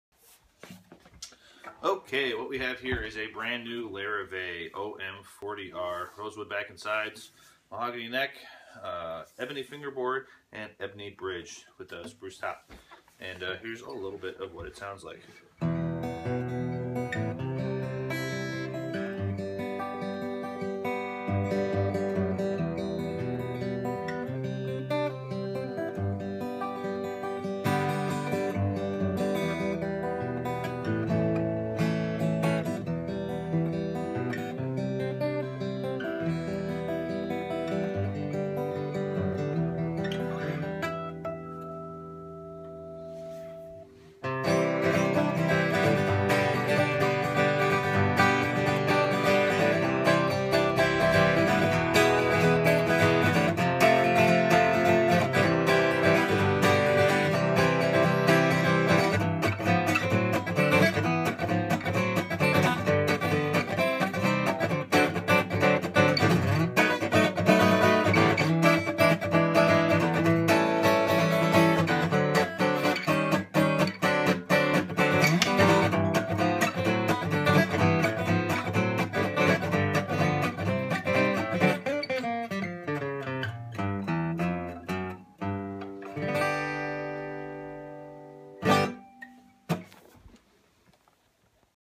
Larrivee OM-40R Demo at Collar City Guitars, Troy NY
Enjoy this little demo to hear how the New Larrivee OM-40R sings.

The OM-40 is a revolutionary new guitar from Larrivee. This new model born from years of research and testing features Jean Larrivee’s first new bracing pattern in over 46 years. The new “Scalloped Parabolic Hybrid” bracing system perfectly merges the strength, tonal balance, and clarity of Larrivee bracing, with the Bass response, depth, and volume you’d expect from a vintage dreadnaught.

Made only in America, the OM-40 comes standard with an exceptional feature set compared to similar style instruments from other manufacturers. The OM-40 has a solid African Ebony Fretboard & Bridge, single piece Mahogany neck, Canadian maple binding, bone saddle, bone nut, and bone bridge pins. Other features we include are a Mother of Pearl Logo, Grover 18:1 open back tuners, non-shrink tortoise pick guard, double action truss rod (easily adjusted at full string tension), re-enforcement for side mount electrics, durable acrylic satin finish, bold rope purfling, arch top case, and a lifetime warranty!